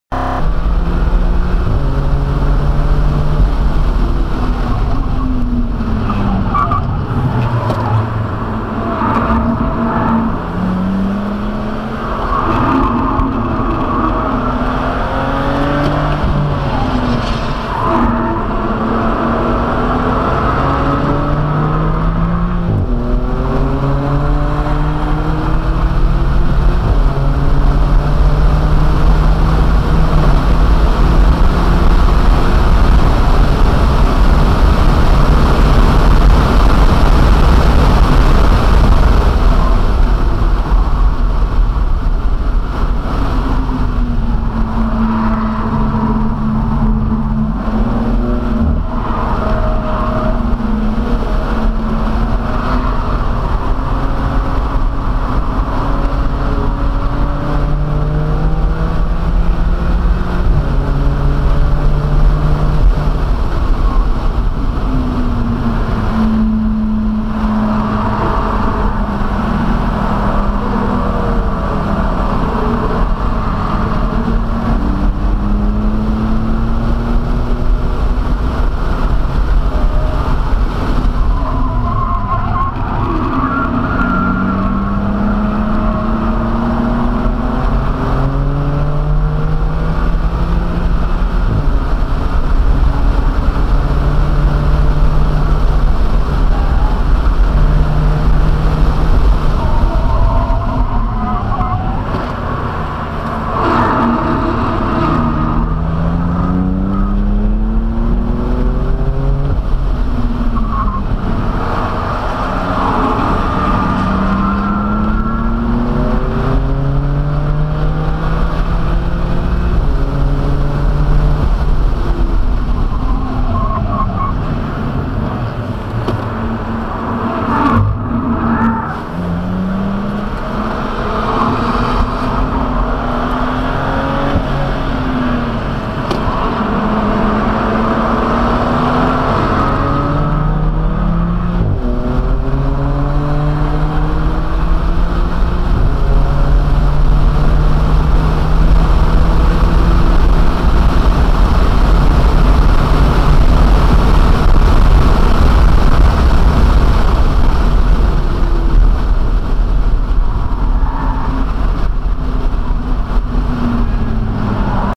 M2 Competition First Outing - The Ridge Motorsports Park
Best time of the day 1:58:09 on stock tires. 12-5-20 Turn 2 HPDE Winter Special